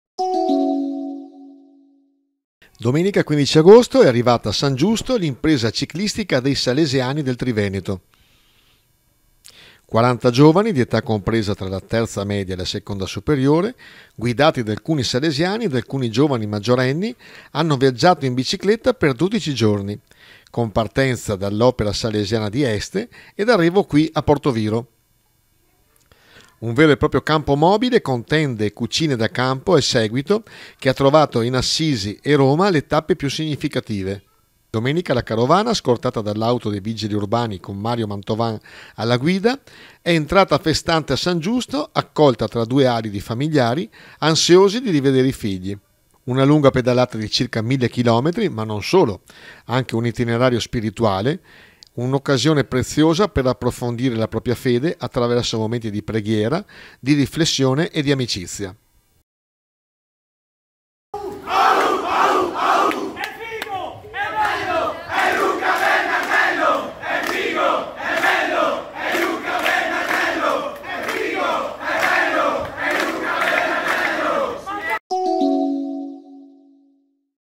Porto Viro - E’ arrivata San Giusto l’Impresa Ciclistica dei salesiani del Trivento
Domenica 15 agosto nel tardo pomeriggio è arrivata all’Oratorio Salesiano san Giusto l’“Impresa Ciclistica”. Quaranta giovani di età compresa fra la terza media e la seconda superiore, guidati da alcuni Salesiani e alcuni giovani maggiorenni hanno viaggiato in bicicletta per 12 giorni